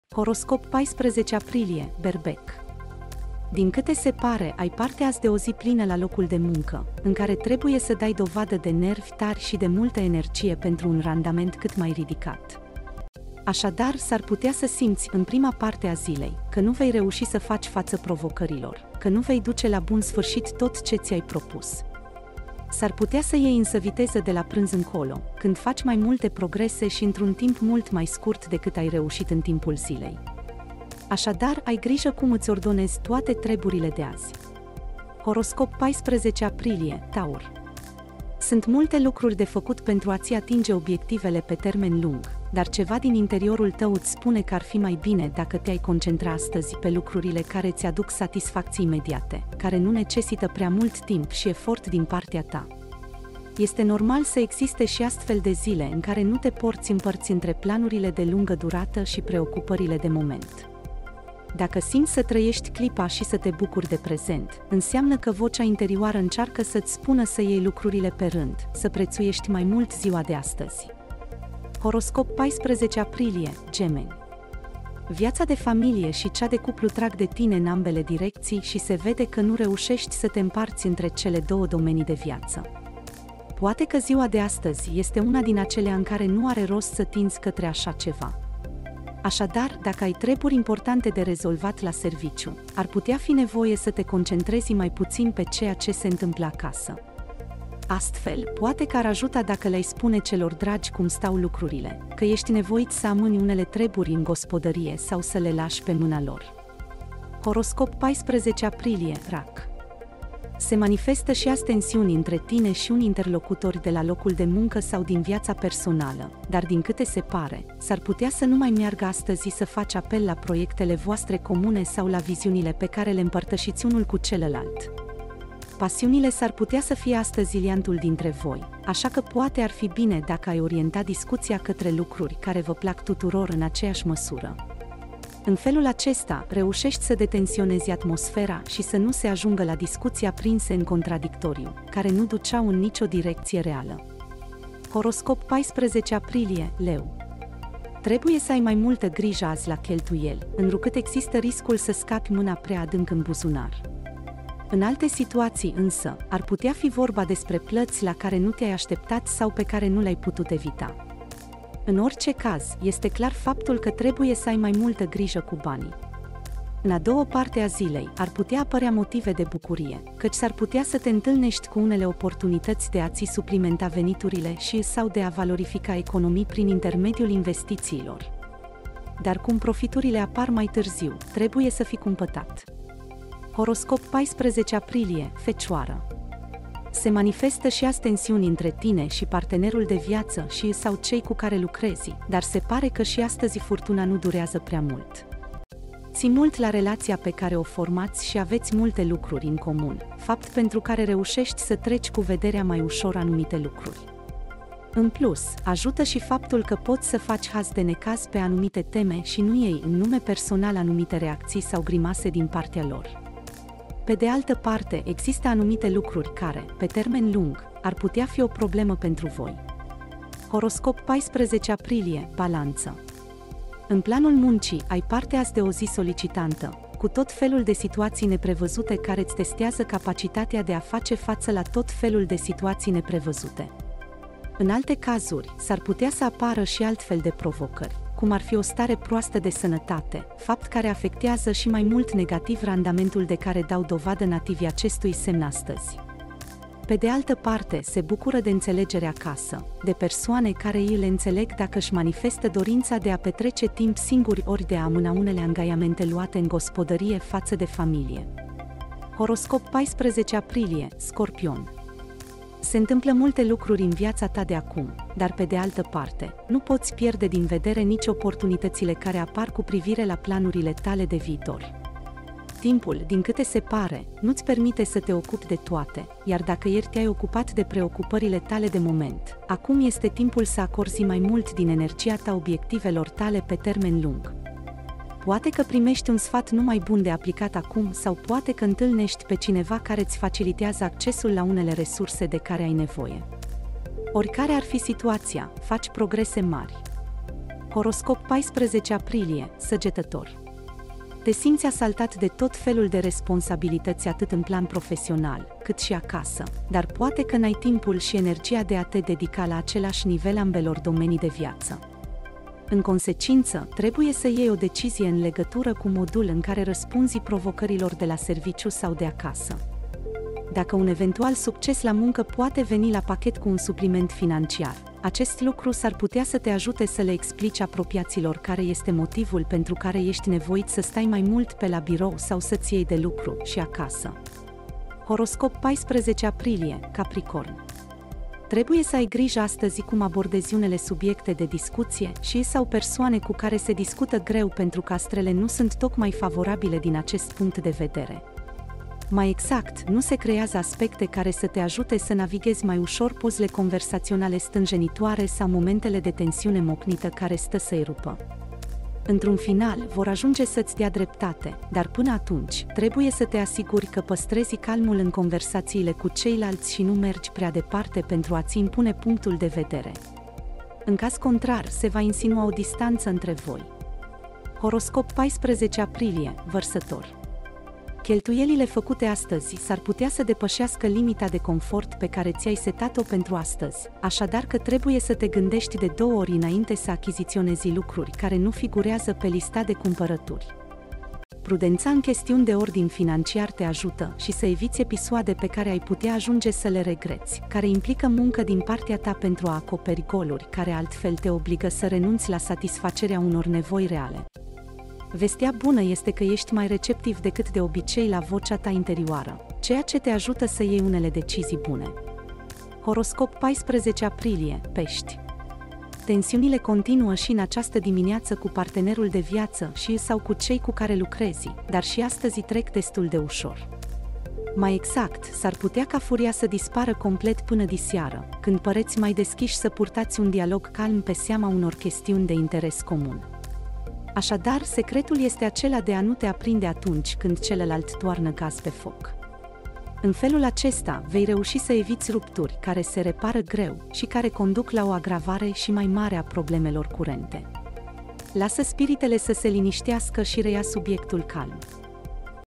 Horoscop pentru 14 aprilie. Astăzi este ultima șansă de a lua decizia mult așteptată. Altfel, soarta
Luna în Fecioară formează o dublă conjuncție cu Neptun și Jupiter în Pești, apoi formează un trigon armonios cu Pluto în Capricorn. A luat stăpânire din atmosfera de ieri, cu semnele mutabile corectate (Gemeni, Fecioară, Săgetător, Pești) simțind mai acut că trebuie să ia decizii impuse pe termen lung.
Horoscop 14 aprilie - Berbec

Evident, ai o zi plină la serviciu și trebuie să arăți nervi puternici și multă energie pentru a obține profituri mari. Drept urmare, în prima jumătate a zilei, s-ar putea să simți că nu poți face față provocării și nu poți face toate lucrurile pe care ai plănuit să le faci. Dar poți lua viteză de la prânz, când faci mai multe progrese și ai ore mai scurte decât în ​​timpul zilei. Așa că ai grijă cum îți faci toate treburile astăzi.

Horoscop 14 aprilie - Taur

Sunt multe de făcut pentru a-ți atinge obiectivele pe termen lung, dar ceva în interiorul tău îți spune că nu este nevoie de mult dacă te concentrezi astăzi pe lucruri care îți oferă timp și energie instantanee pentru satisfacție. Partea ta. Sunt zile în care este normal să nu faci diferența între planurile pe termen lung și grijile actuale. Dacă vrei să trăiești momentul și să te bucuri de momentul, înseamnă că vocea ta interioară încearcă să-ți spună să te iei mai ușor și să apreciezi mai mult ziua.

Horoscop 14 aprilie - Gemeni

Viața de familie și cea de căsătorie te atrag în ambele direcții și este clar că nu te poți împărți între cele două domenii ale vieții tale. Poate că astăzi este una dintre acele zile care nu are sens. Așadar, dacă aveți o muncă importantă de făcut la serviciu, poate fi necesar să acordați mai puțină atenție la ceea ce se întâmplă acasă. Așa că ar putea ajuta să le spuneți celor dragi cum trebuie să amânați unele treburi sau să le predați lor.

Horoscop 14 aprilie - Rac

În viața ta profesională sau personală, există încă tensiuni între tine și unii dintre interlocutorii tăi, dar evident, s-ar putea să nu poți să-ți folosești proiectele sau viziunile comune pe care le împărtășești într-o zi cu alta. Pasiunea poate fi legătura dintre voi astăzi, așa că poate fi o idee bună să vă concentrați pe ceva ce vă place amândoi. În acest fel, poți să relaxezi atmosfera în loc să ai o discuție conflictuală aprinsă care nu duce la nicio direcție reală.

Horoscop 14 aprilie - Leu

Când vine vorba de cheltuieli, trebuie să fii mai atent, deoarece mâinile tale pot pătrunde prea adânc în buzunare. Cu toate acestea, în alte cazuri, poate fi o plată cu care nu sunteți de acord